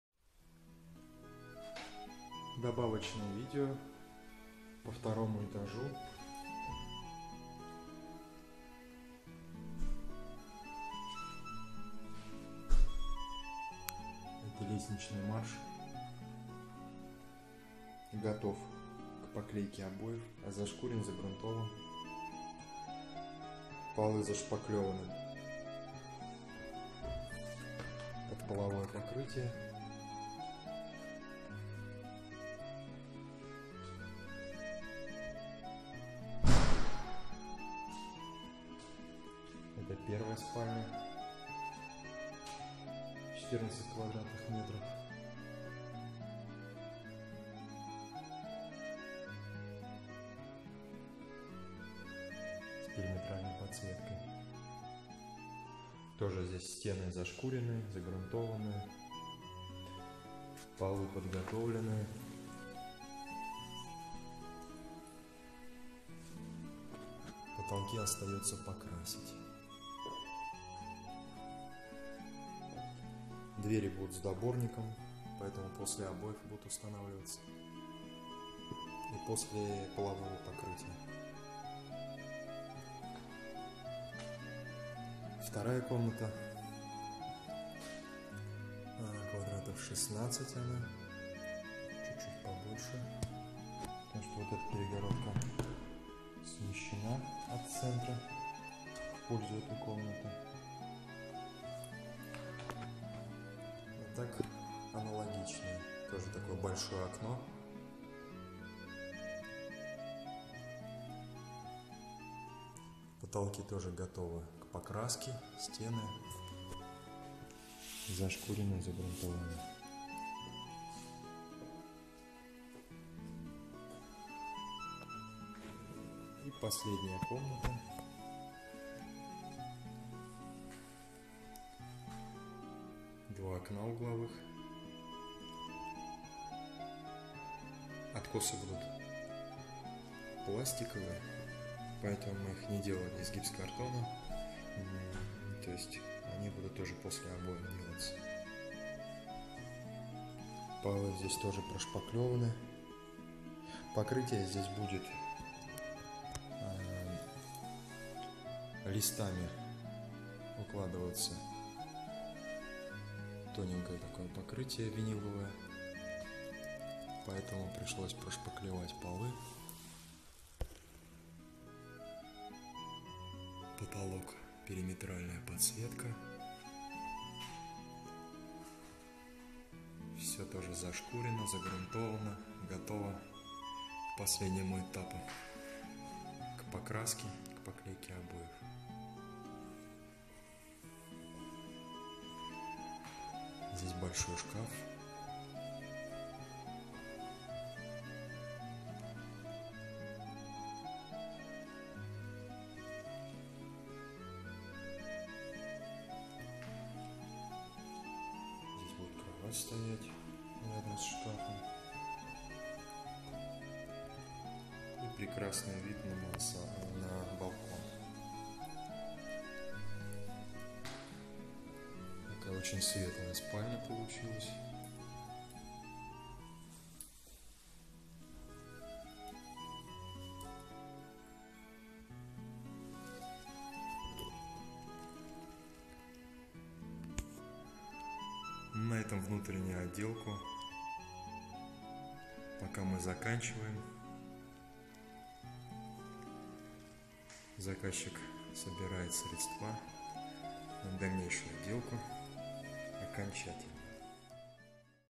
Шпаклевка в каркасном доме. Добавочное видео.frame house. Каркасный дом 8/9
Моя партнерская программа для канала шпаклевка в каркасном доме. Подготовка под обои, покраску и настил венилового ламината. Дом 8/9 второй этаж. frame house.